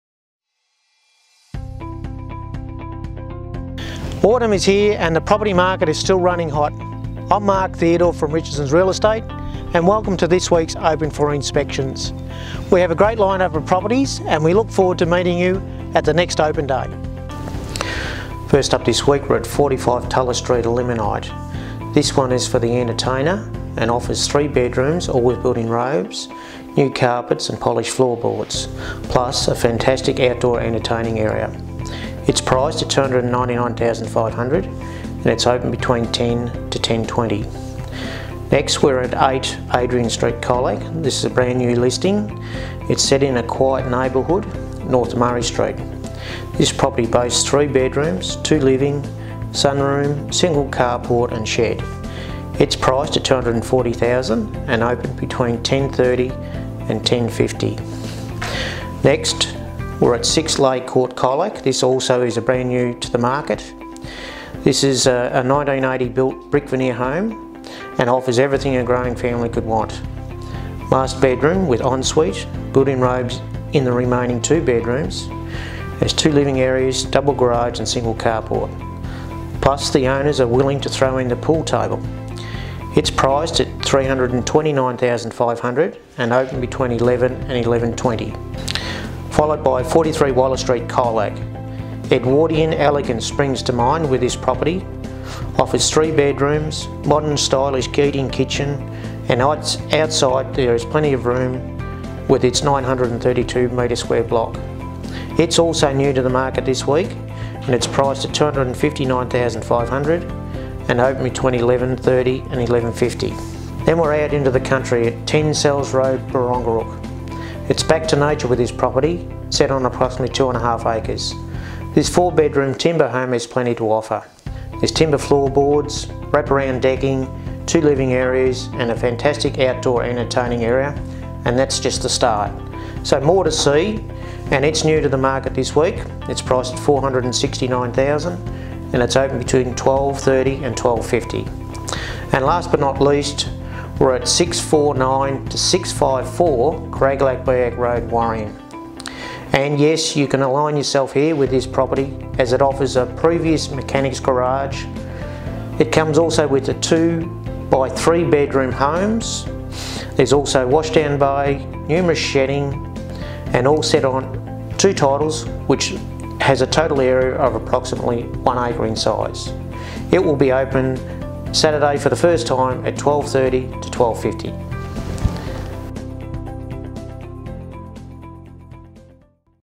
Open for inspection 4th of March
We have a fantastic line up of properties open for inspection this weekend, we look forward to seeing you there!